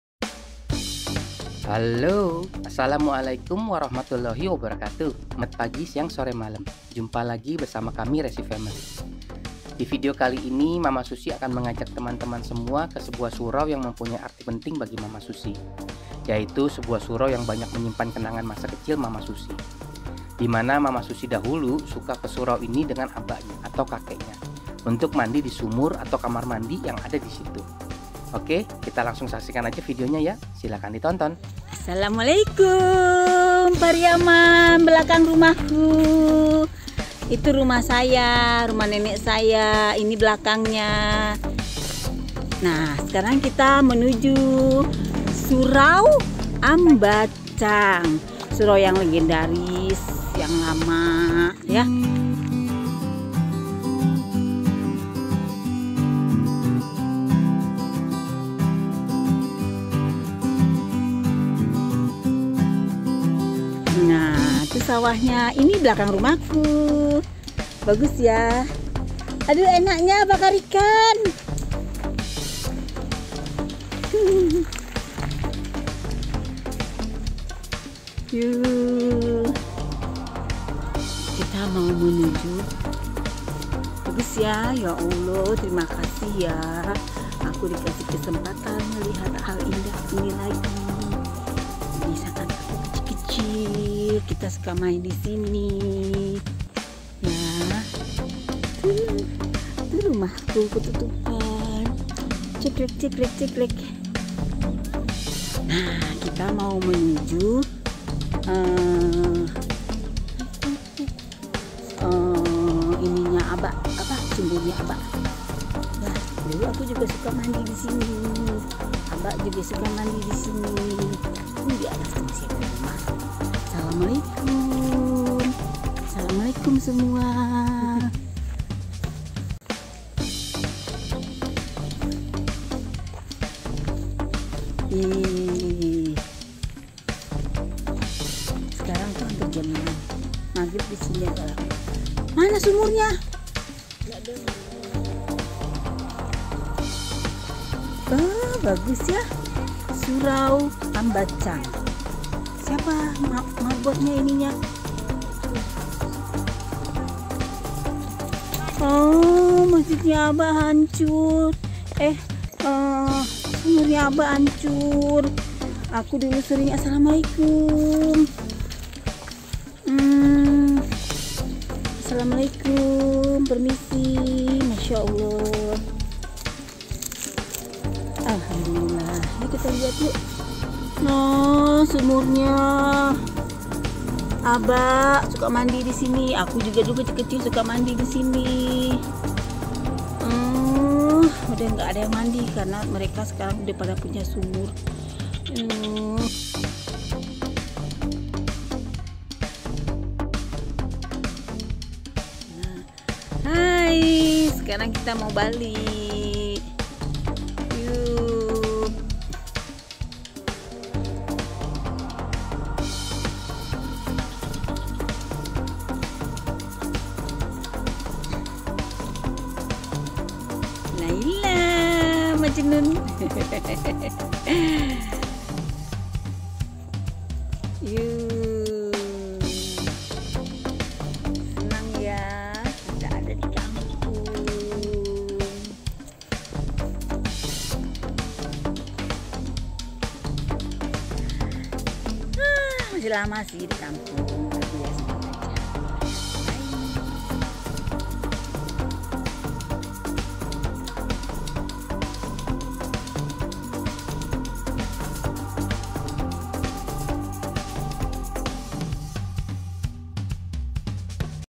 Sumur Kenangan di Surau Ambacang yg Legend di Kampung Dalam.
Menuju Surau yg terdapat Sumur yg penuh kenangan bagi mama Susi